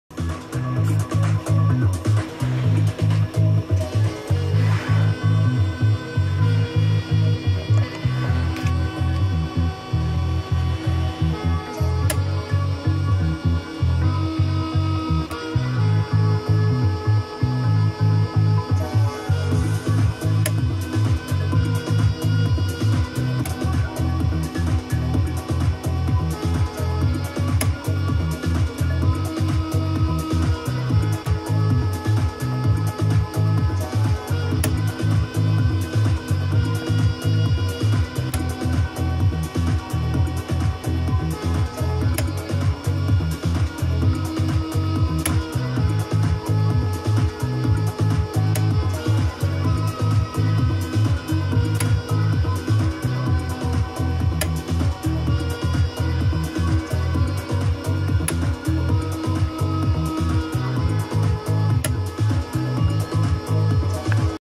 Nakamichi 680ZX Source vs Tape Monitor - SA90 Normal Speed
This is a Nakamichi 680ZX tape deck recording at standard speed (4.76 cm/sec) on a TDK SA90 High Position tape. Notice the brighter treble between the tape signal (monitor switch on the left) and the incoming source (right)